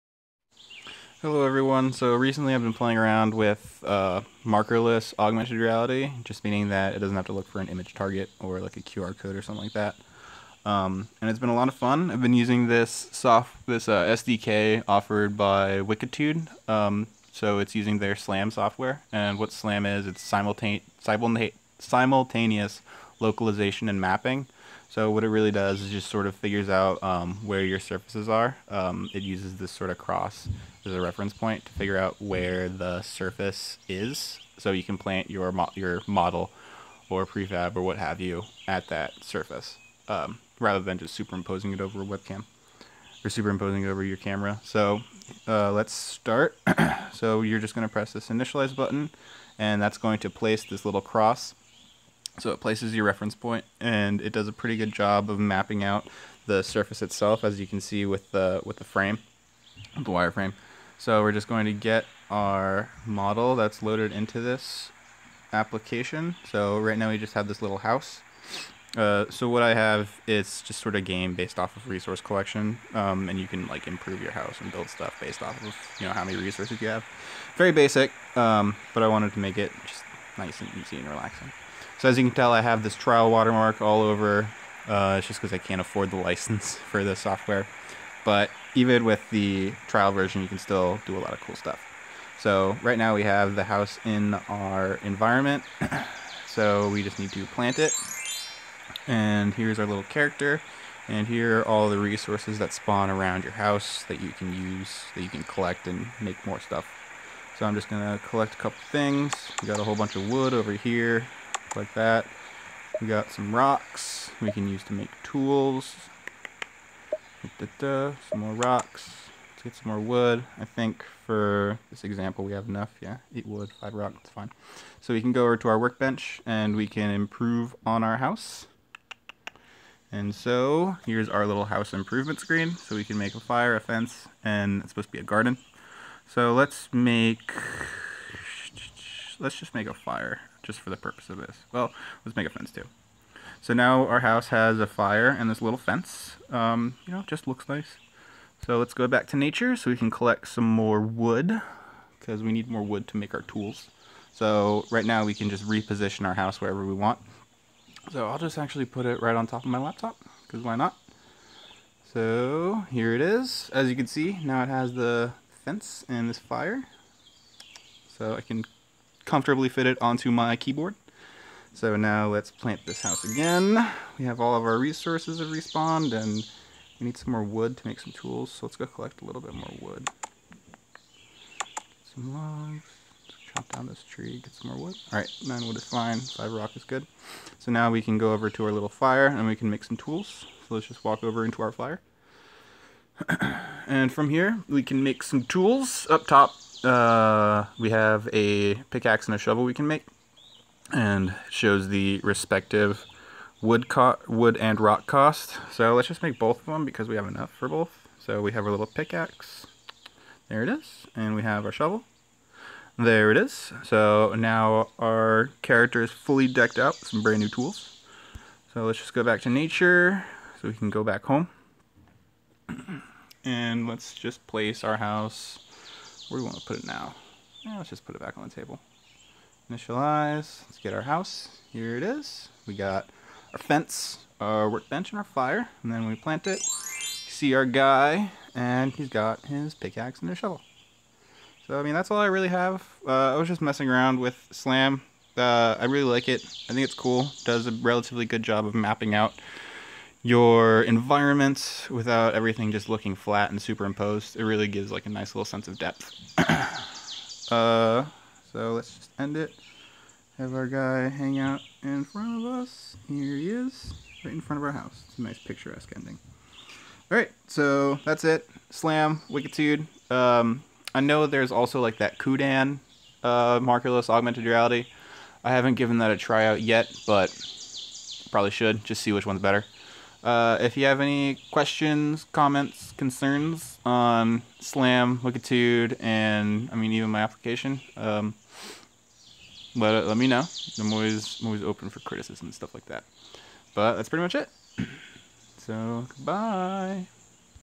Using Wikitude SLAM on mobile devices - Augmented Reality (AR / MR)
I made a small AR mobile game for Android using SLAM (simultaneous localization and mapping). It does a great job of adding depth to your AR app without using an image target or QR code.